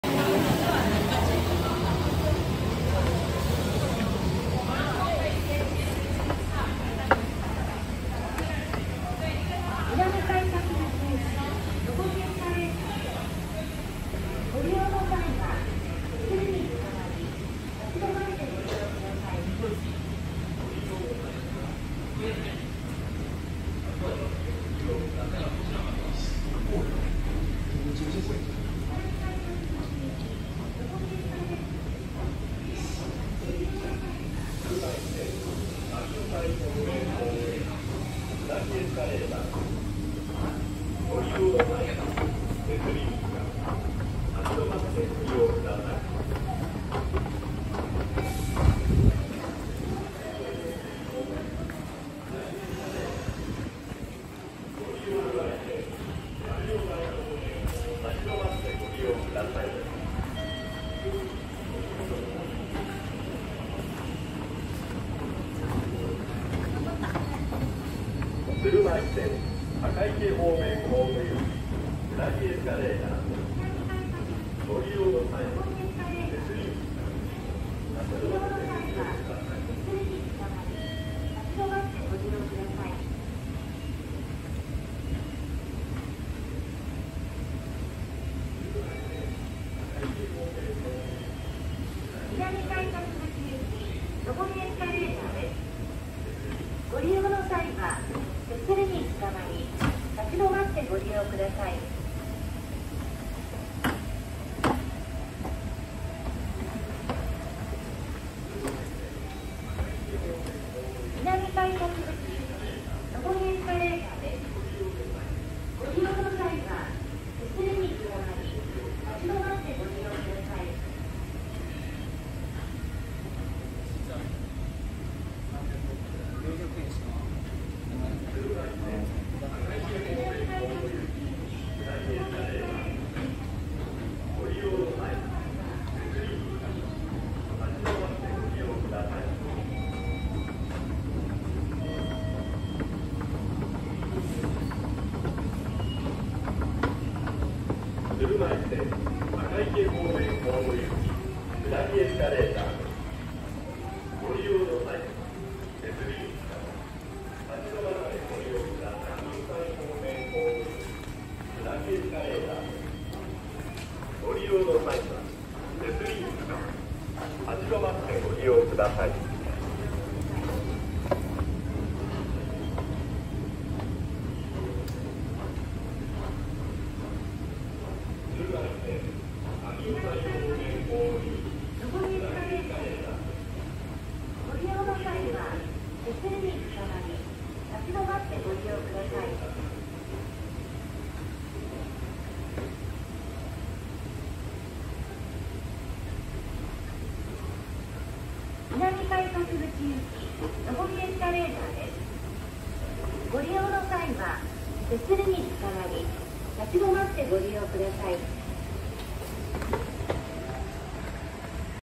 【日立】2X エスカレーター 伏見駅南改札内 2X escalator Fushimi station Nagoya City subway Aichi Japan
伏見駅南改札内にあるエスカレーターです。

乗り物だいすきな方は、こちらも是非ご覧ください　「乗り好き トレイン」